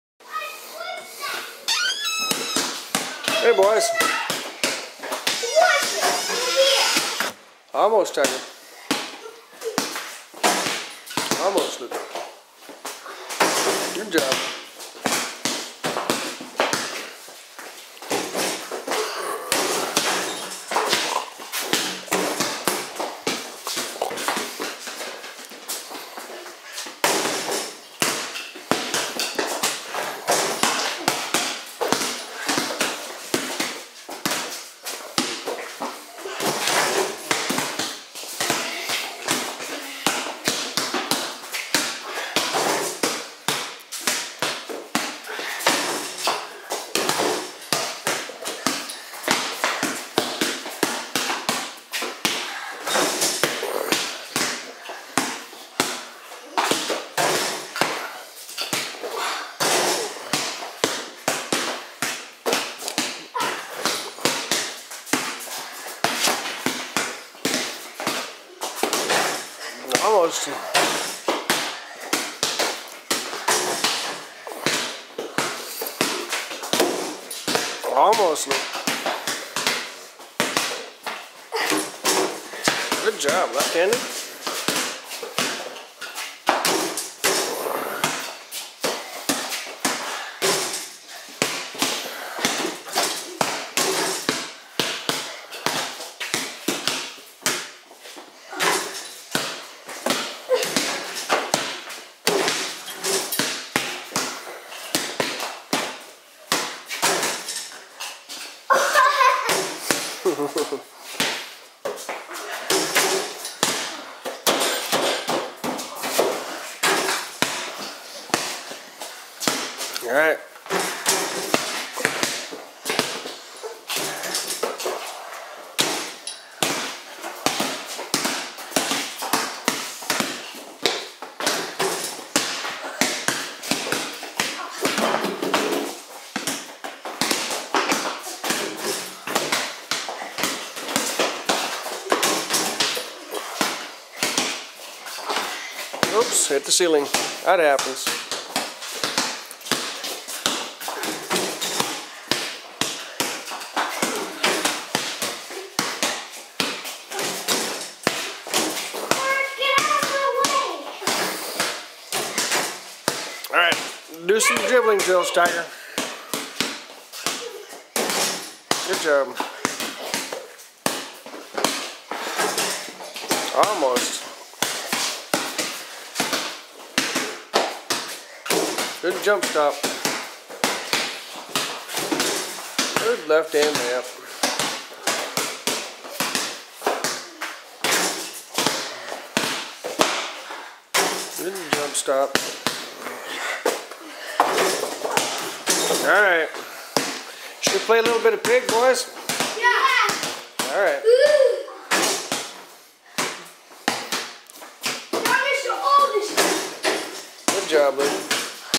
Mark (3rd) & Luke (1st) in the garage.MTS
The boys like to shoot hoops in the garage during the winter. We bought a SKILZ miniture basketball hoop to fit in our 9'6" high garage and a few youth basketballs. The goal is at 6'8" and the distance from the hoop to the garage door is 17'.